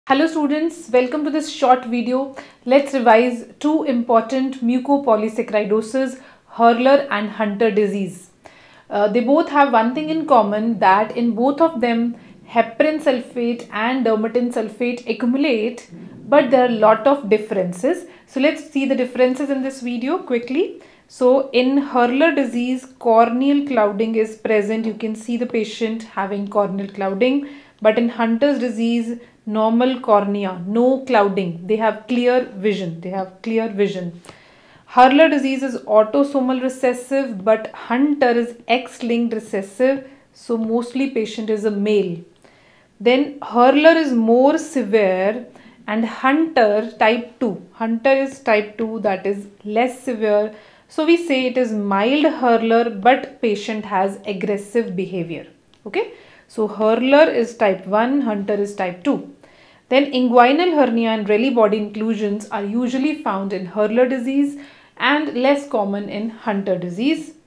Dr. Smily Discusses the "Differences between Hurler and Hunter Disease"
To study Biochemistry with over 19,000 other students and get access to more free videos, discussions, and updates join our communities:-